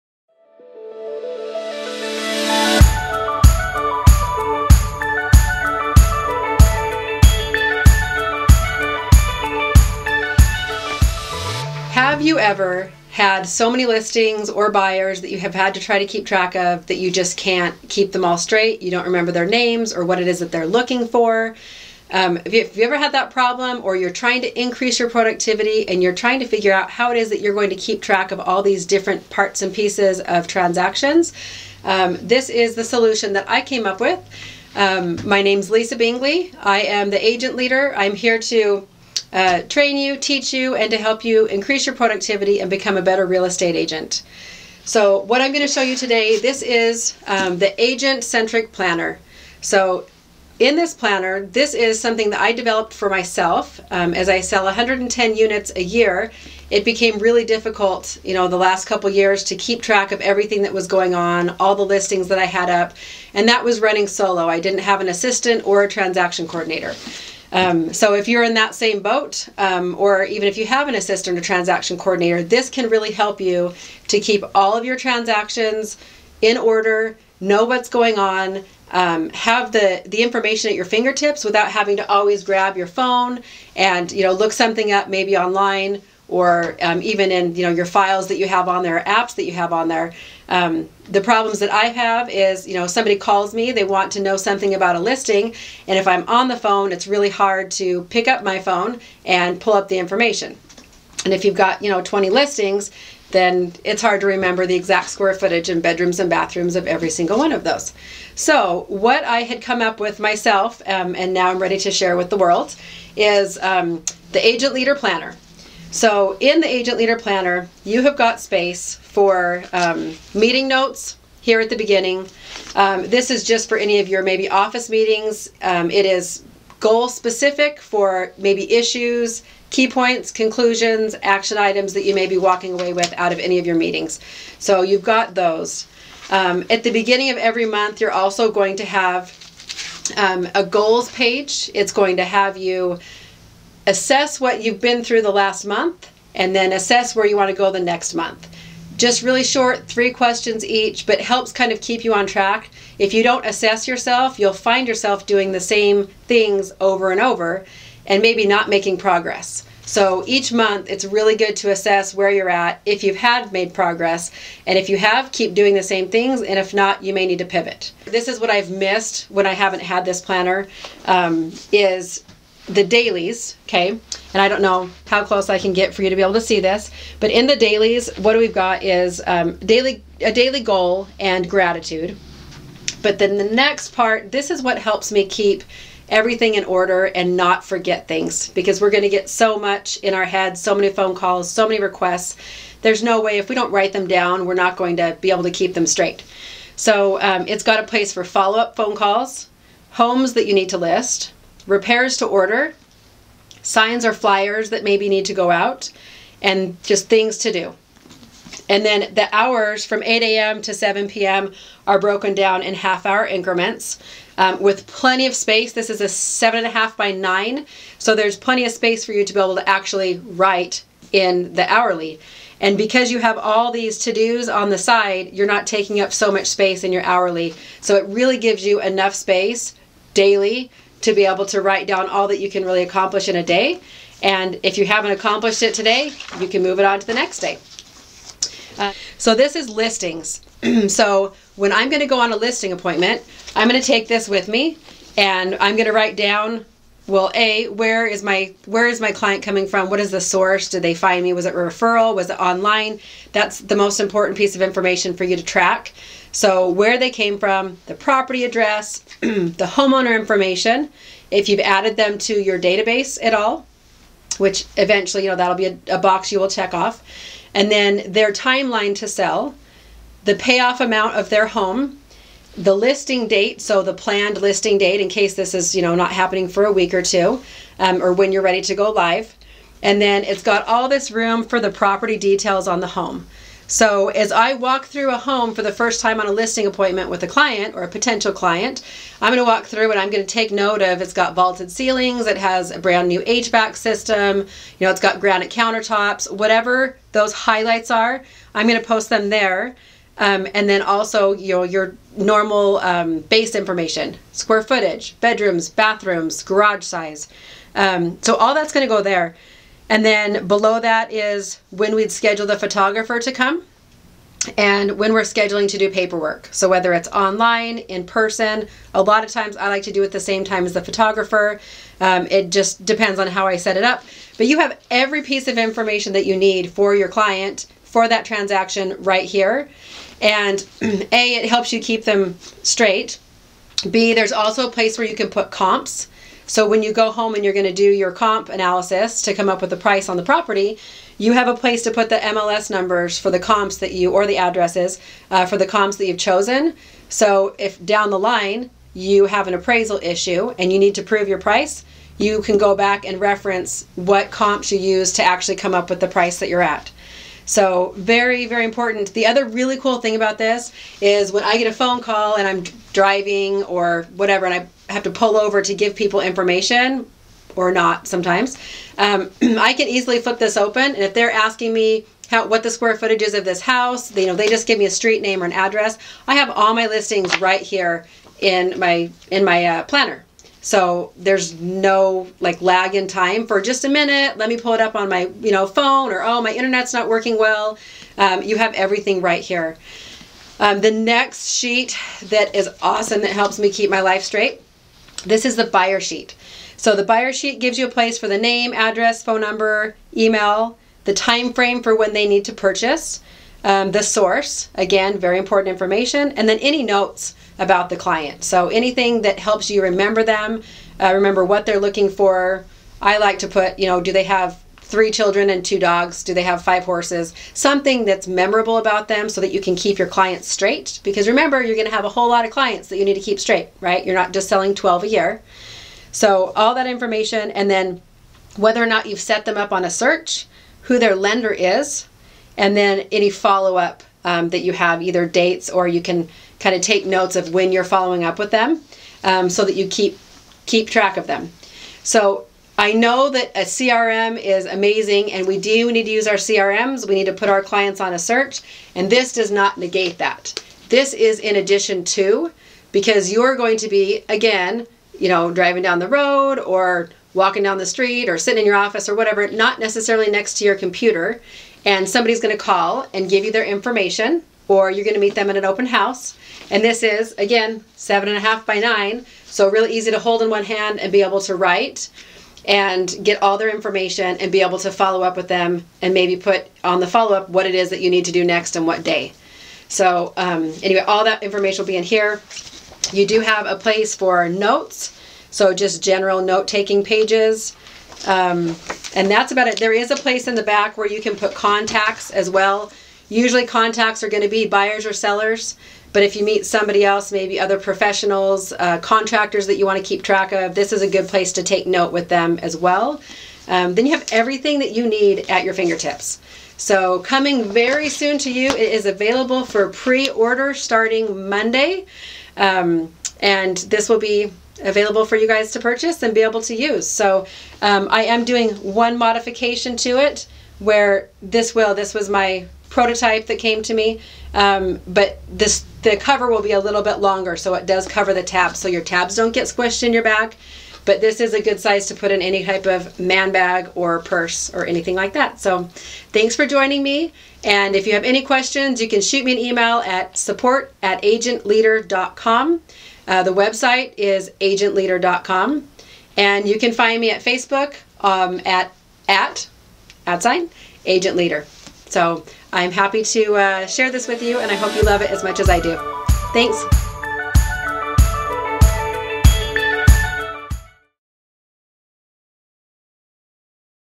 Agent Leader Real Estate Planner Overview
The perfect size planner, 7.5 x 9 spiral bound fits nicely in your hand to walk through listings and take notes, plus it fits in your bag or purse. Daily pages are designed to help you remember all your upcoming tasks with prompt lines including follow-up calls, homes to list, repairs to order, signs/flyers, things to do and all while allowing room for half hour increment scheduling 8am-7pm. Weekly planning page, monthly calendar spread, meeting notes, marketing plan, goal setting and monthly assessment. And my favorite? Listing pages and buyer sheets so you can keep all the important information you need in the palm of your hand and when someone calls, you’ll be quick to respond! The best tool to keep yourself organized and improve productivity.

On a mission to become a top agent, but can’t seem to keep track of ALL the to-dos?
Follow-up calls, homes to list, repairs to order, signs/flyers, showings, contracts. And the list goes on. 🥵
When I started my journey as a realtor I was in the same boat.
And I couldn’t for the life of me find a planner that was geared towards realtors . . . and didn’t cost a fortune.
So I created an all-in-one solution to help myself and other realtors keep track of:
📝 Daily objectives with prompts (no more sinking “I forgot something” feelings!)
📝 Meeting notes & Goal setting
📝 6-month marketing plan
📝 AND handy listing pages and buyer sheets
Plus, it tucks inside bags and purses. Because we’re not just on-the-go agents, we’re go-getters! 🏡